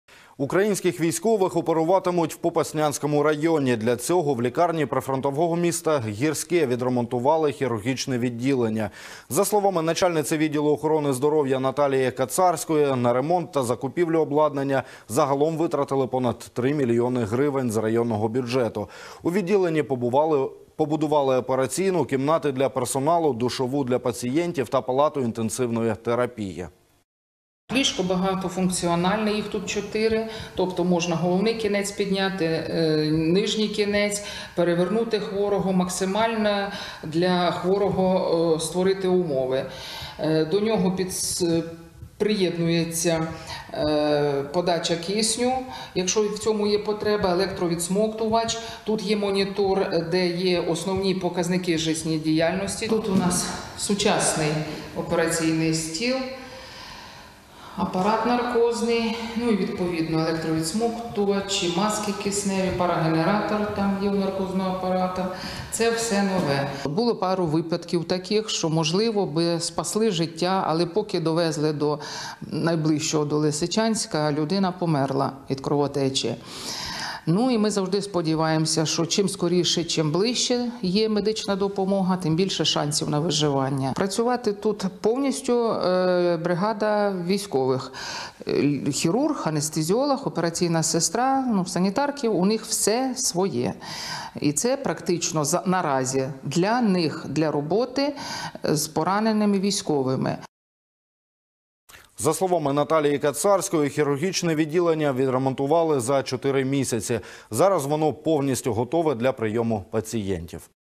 Хірургічне відділення відремонтували у Попаснянському районі
Українських військових оперуватимуть в Попаснянському районі. Для цього в лікарні прифронтового міста Гірське відремонтували хірургічне відділення. За словами начальниці відділу охорони здоров’я Наталії Кацарської на ремонт та закупівлю обладнання загалом витратили понад 3 мільйони гривень з районного бюджету. У відділенні побудували операційну, кімнати для персоналу, душову для пацієнтів та палату інтенсивної терапії.